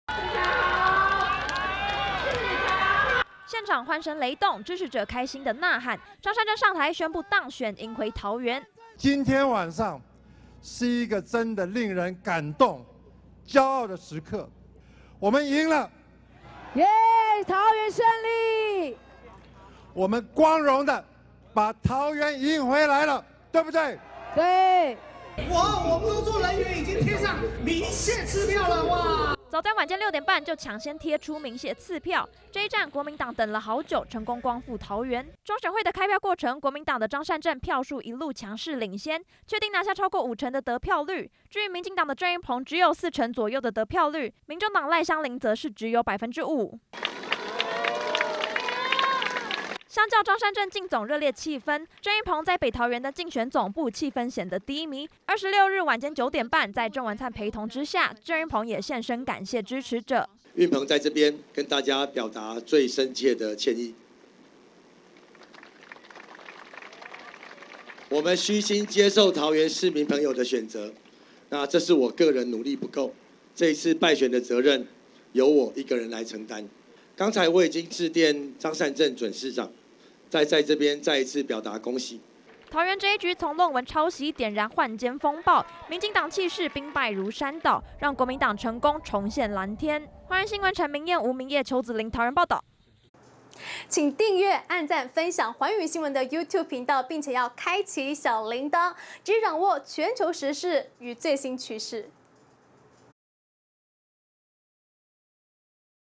國民黨桃市票拿過半 張善政高喊：人民勝利@globalnewstw
國民黨張善政自行宣布當選 桃園市長，在發表勝選感言時，他說這個勝利，不是個人的勝利，是屬於全桃園人民的勝利。還高喊，光榮的把桃園贏回來，最終拿下了過半得票率，成功光復藍天。至於，民進黨的鄭運鵬在鄭文燦的陪同下感謝支持者，也坦承努力不夠虛心接受市民選擇。